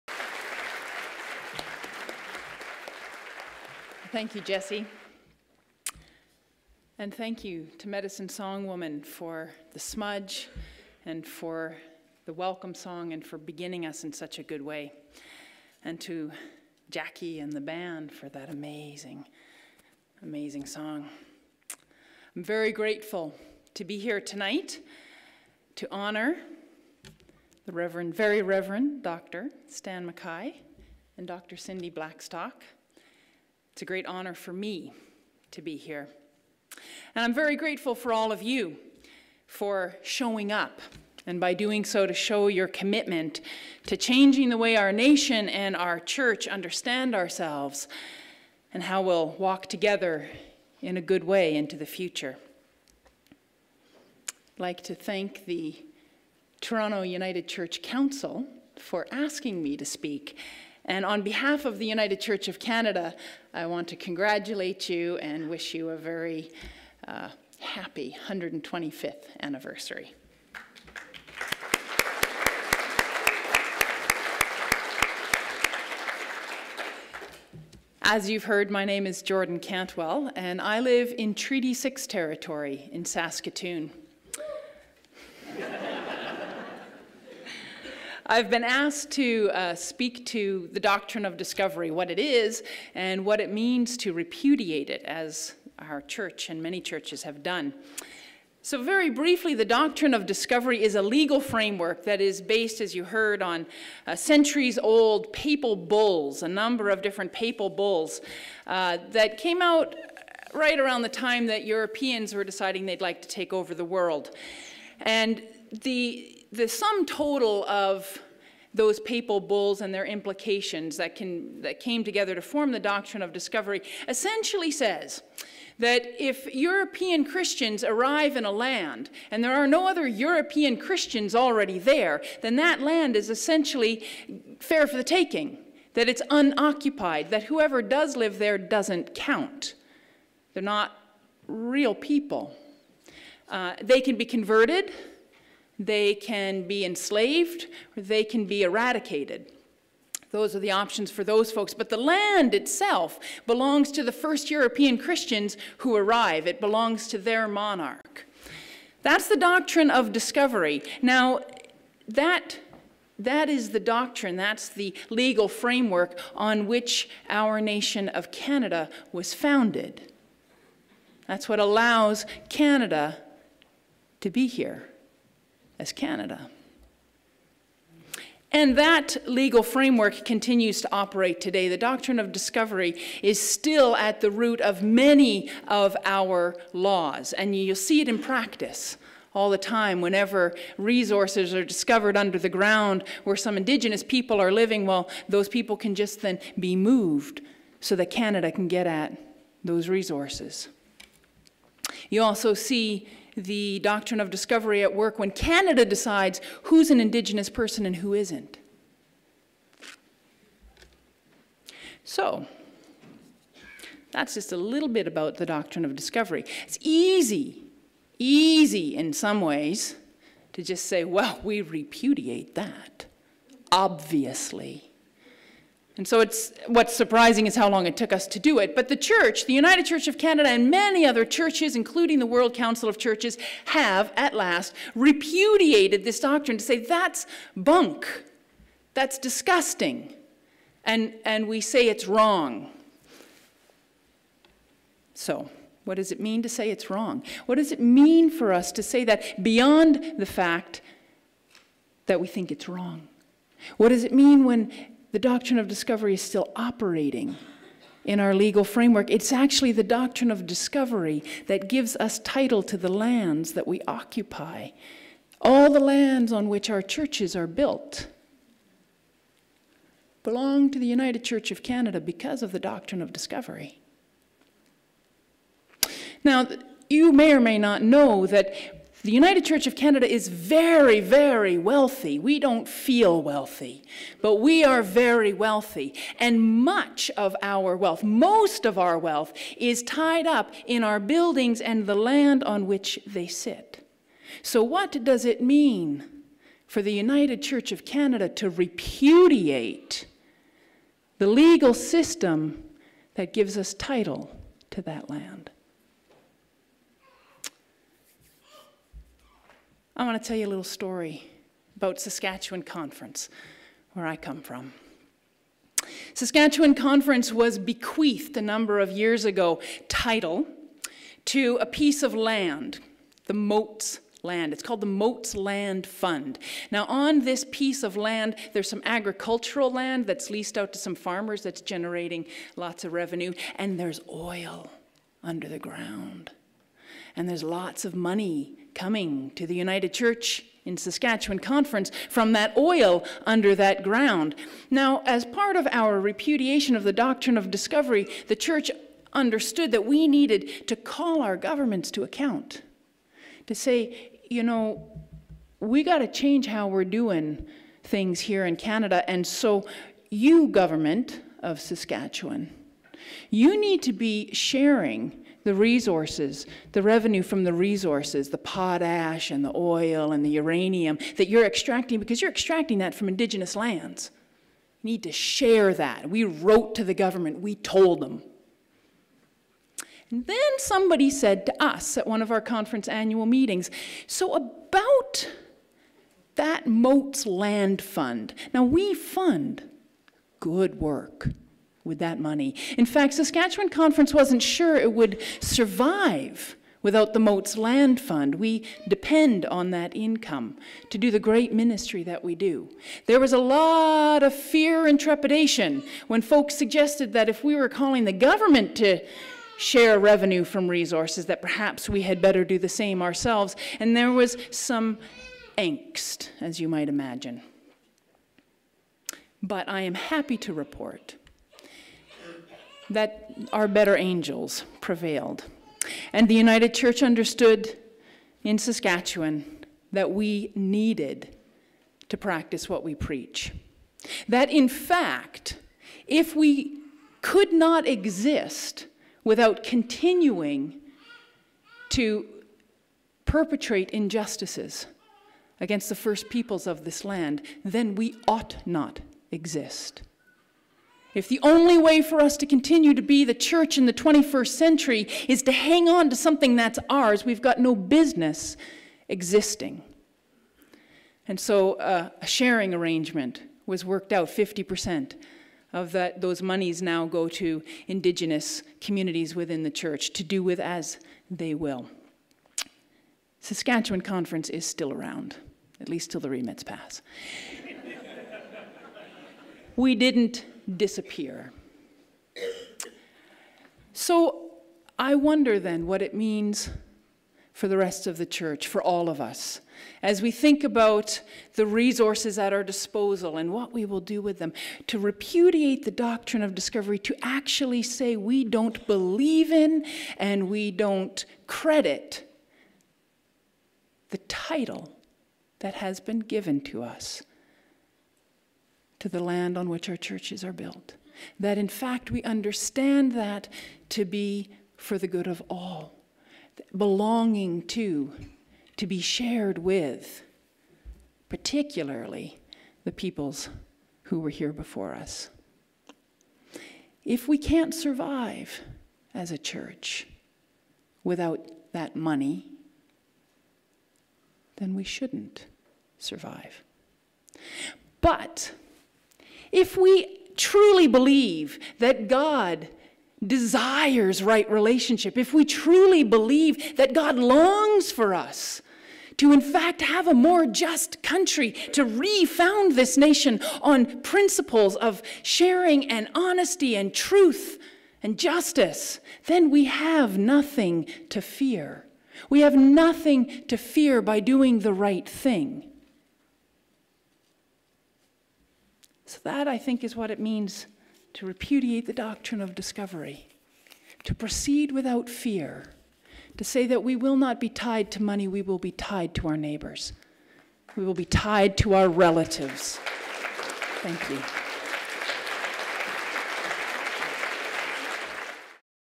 The Right Reverend Jordan Cantwell Heart & Vision 2017
Council's 9th annual Heart & Vision Awards concert on May 15 was an unforgettable evening focused on reconciliation between First Nations and Canadians of settler heritage. Dr. Cindy Blackstock and the Very Rev. Stan McKay were honoured for their advocacy for First Nations peoples. The Right Reverend Jordan Cantwell, Moderator of the United Church of Canada, spoke about the Doctrine of Discovery. The concert raised over $9,000 for Council's Community Relief Fund.